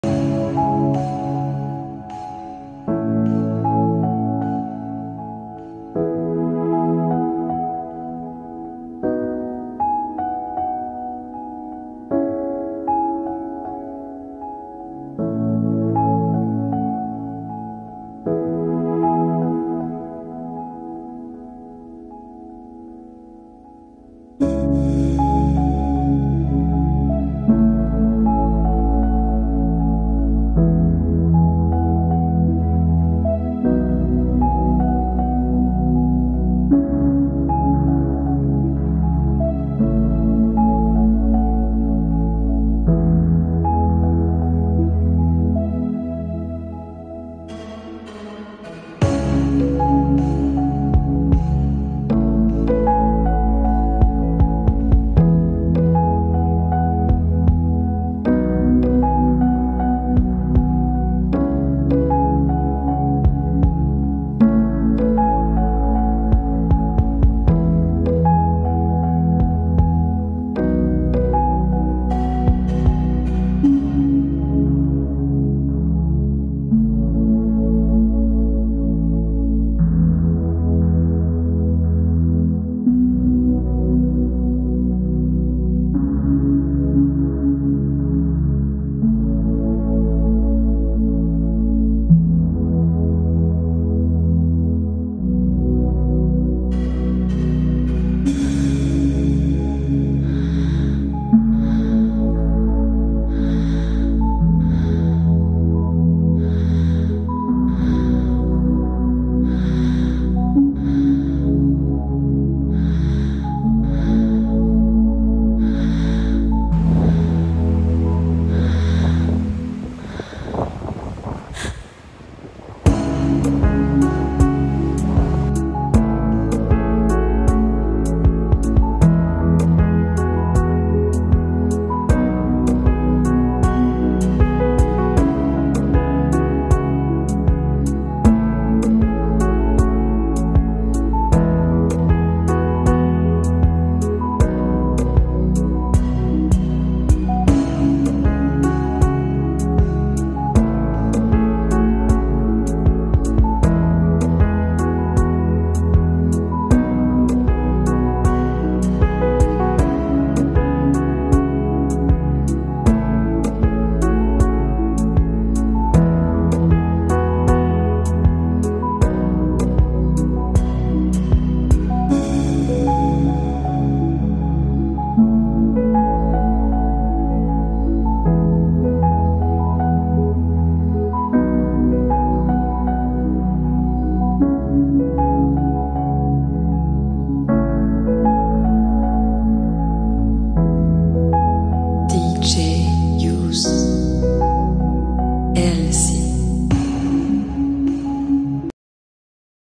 amanecer volando entre nubes dron DJI mini 2 pajaros añadidos 4K Lousada Lugo kizomba DjYouss&Ellsys
amanecer volando sobre las nubes dron DJI mini 2  con pajaros añadidos po edicion
DJI mini 2 Guntin Lugo 4K Lousada (Guntin) Lugo

musica kizomba Dj Youss & Ellsys